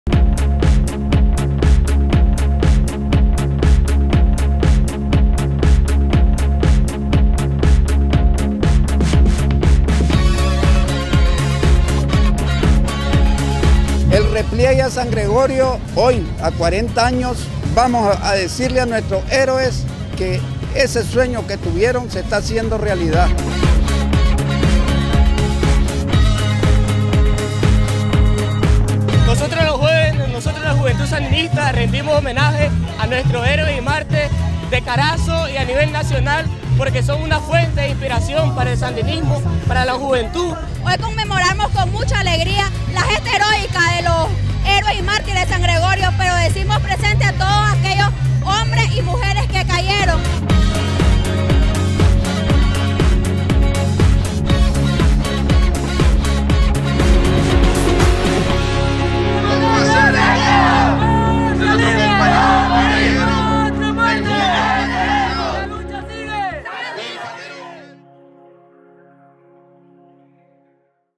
40 Edición del Repliegue a San Gregorio, Diriamba
Familias de Carazo rinden homenaje a los héroes y mártires del Repliegue a San Gregorio

Juventud Presidente